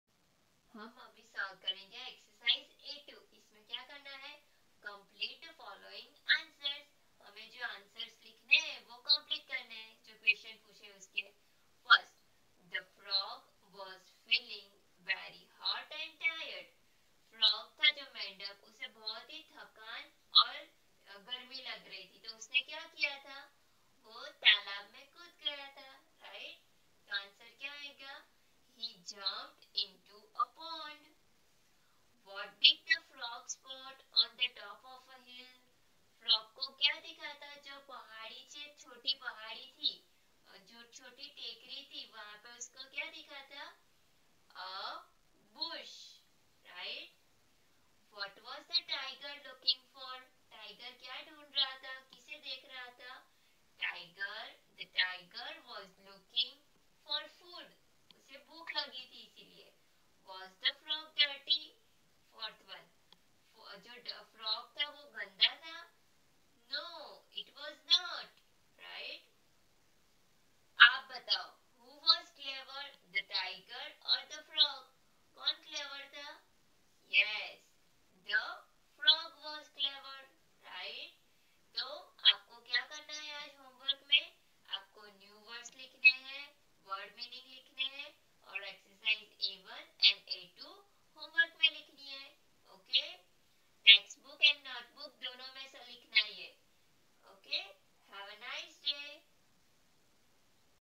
STD 1 Real English (10. The Clever Frog exercise A2)
Standard - 1

Detail explanation about lesson 10( The Clever Frog)
Exercise A2

From the book Real English (A multi - skilled language course ) , 2018 edition